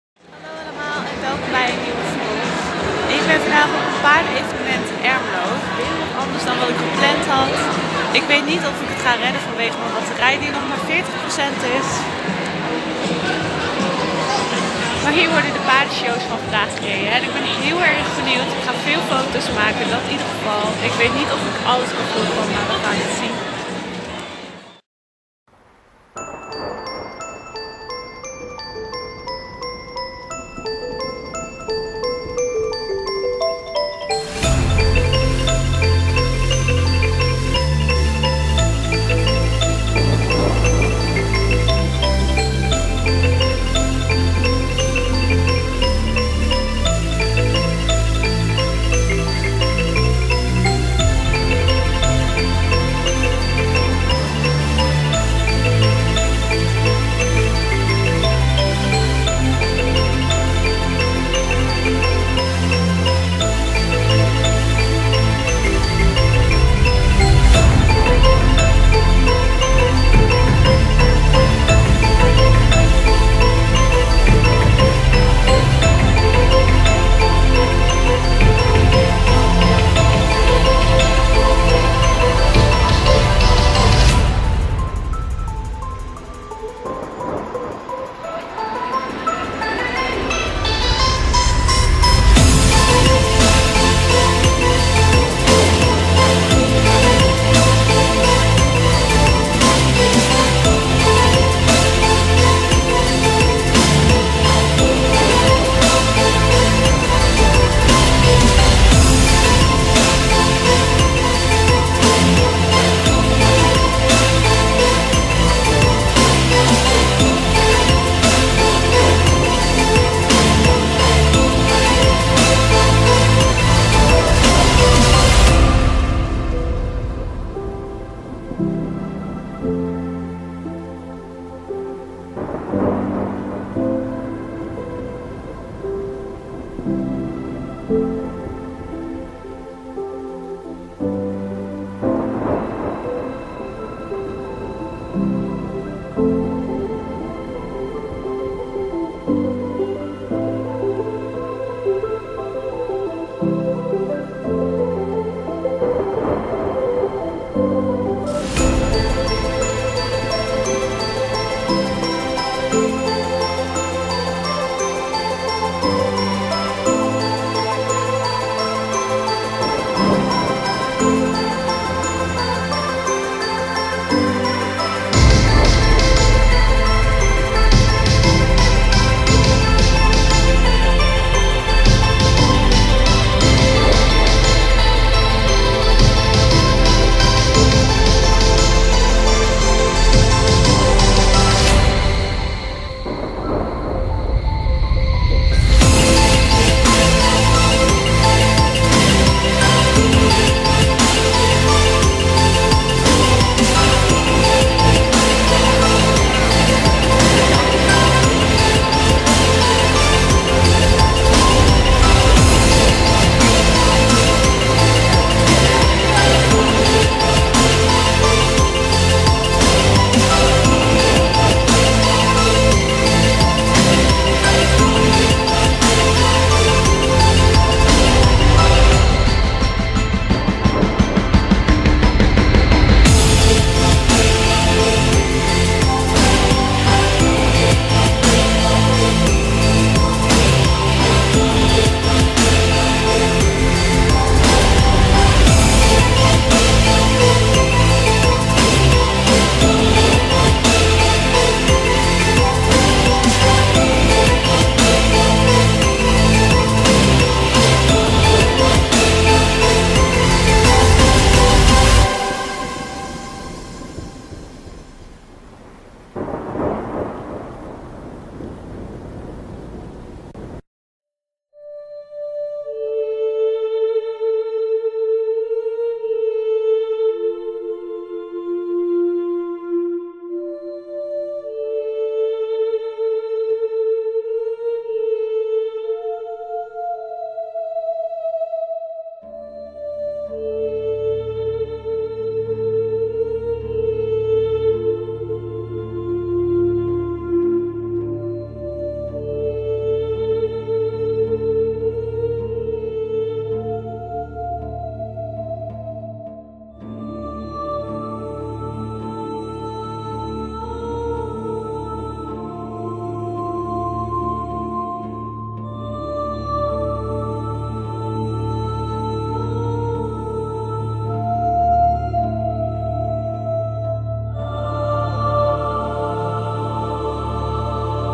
EquiDay Vlog
EquiDay in Ermelo

Voor iedereen die zoekt naar
paardvriendelijke oplossingen en inspiratie.

Mijn telefoon was aan het einde van de dag helemaal leeg geraakt. Ik hoop jullie de vlog leuk vinden.
Het was een interessante dag vol activiteiten over paarden.

Als jullie deze dag ook aanwezig waren. Wat vonden jullie ervan.